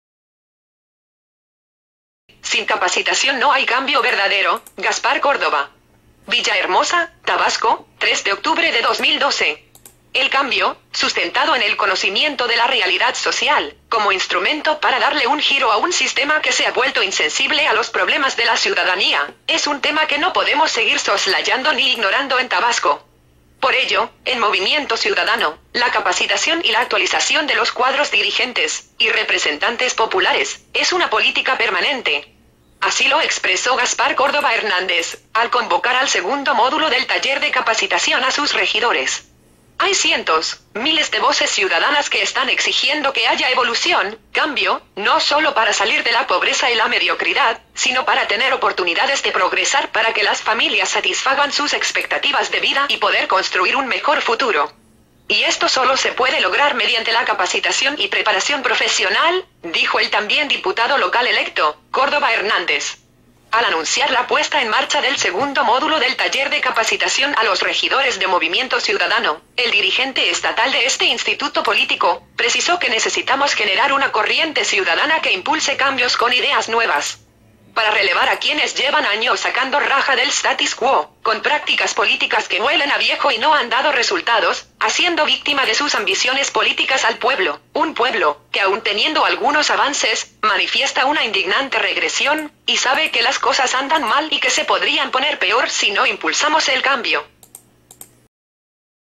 SIN CAPACITACION NO HAY CAMBIO VERDADERO, GASPAR CÓRDOBA.mp4
SIN CAPACITACION NO HAY CAMBIO VERDADERO, GASPAR CÓRDOBA.
Villahermosa, Tabasco, 3 de octubre de 2012.
El cambio, sustentado en el conocimiento de la realidad social,   como instrumento para darle un giro a un sistema que se ha vuelto insensible a los problemas de la ciudadanía, es un tema que no podemos seguir soslayando ni ignorando en Tabasco. Por ello, en Movimiento Ciudadano, la capacitación y la actualización de los cuadros dirigentes, y representantes populares, es una política permanente. Así lo expresó Gaspar Córdoba Hernández, al convocar al segundo módulo del Taller de Capacitación a sus Regidores.
Hay cientos, miles de voces ciudadanas que están exigiendo que haya evolución, cambio, no sólo para salir de la pobreza y la mediocridad, sino para tener oportunidades de progresar  para que las familias satisfagan sus expectativas de vida y poder construir un mejor futuro. Y esto solo se puede lograr mediante la capacitación y preparación profesional, dijo el también diputado local electo, Córdoba Hernández.
Al anunciar la puesta en marcha del segundo módulo del Taller de Capacitación a los Regidores de Movimiento Ciudadano, el dirigente estatal de este instituto político, precisó que necesitamos generar una corriente ciudadana que impulse cambios con ideas nuevas; para relevar a quiénes llevan años sacando raja del status quo, con prácticas políticas que huelen a viejo y no han dado resultados,  haciendo víctima de sus ambiciones políticas al pueblo, un pueblo, que aun teniendo algunos avances, manifiesta una indignante regresión, y sabe  que las cosas andan mal y que se podrían poner peor si no impulsamos él cambio.